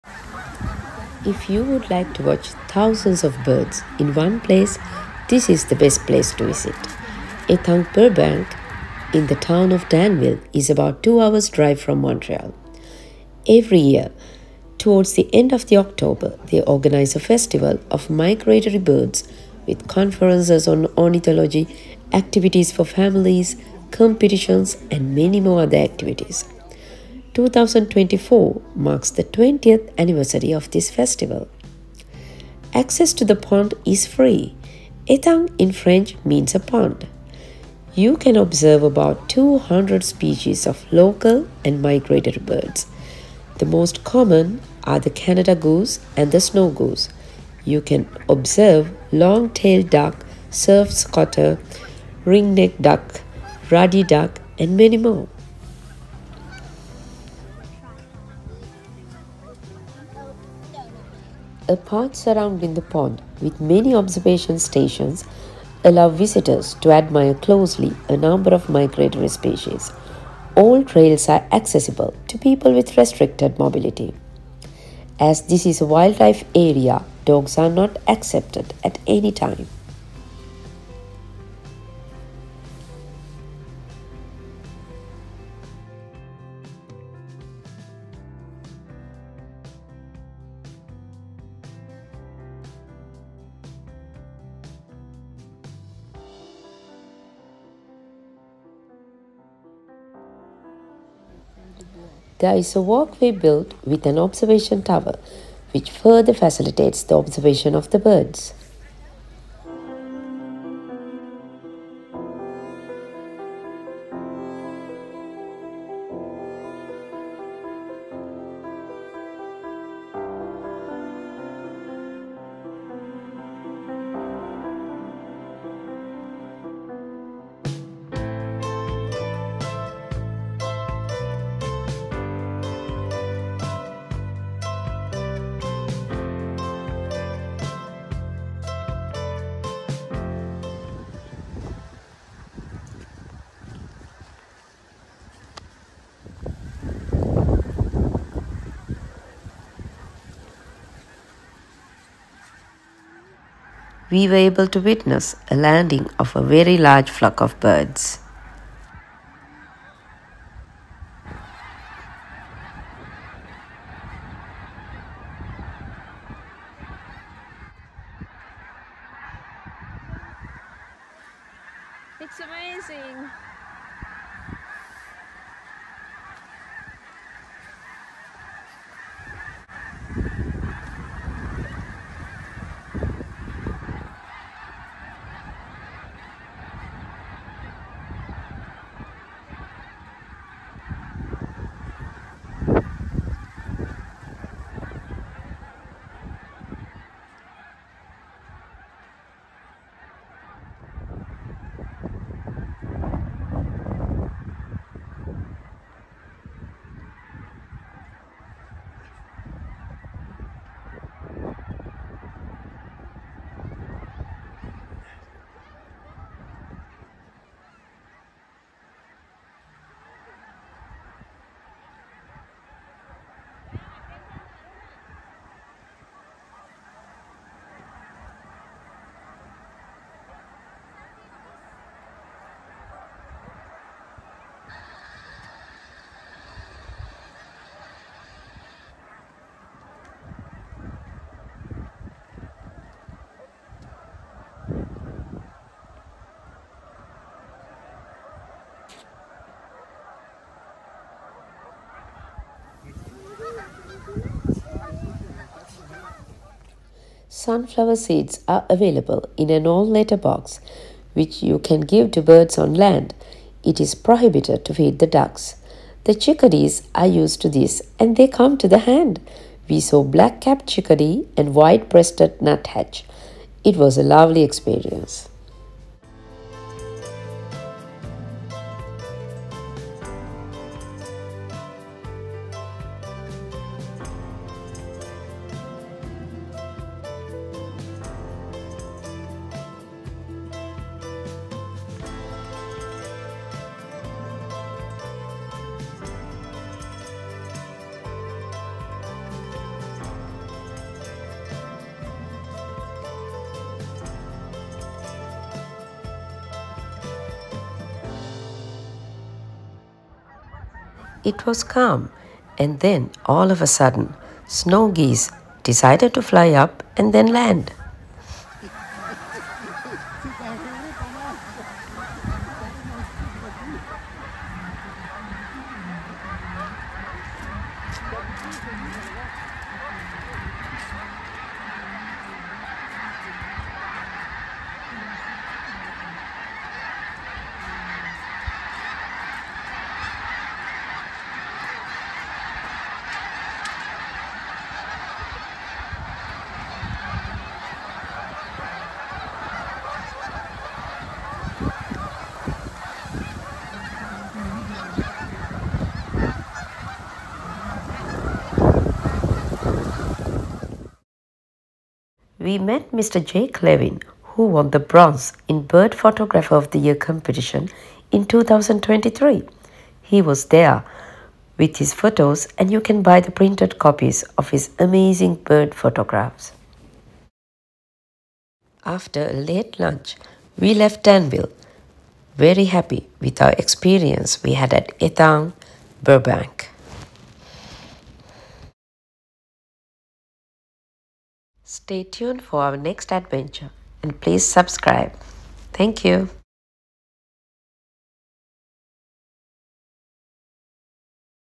Étang Burbank | Best Place to see Migratory Birds | Quebec
Étang Burbank is about 2 hrs from Montreal situated in little town of Danville.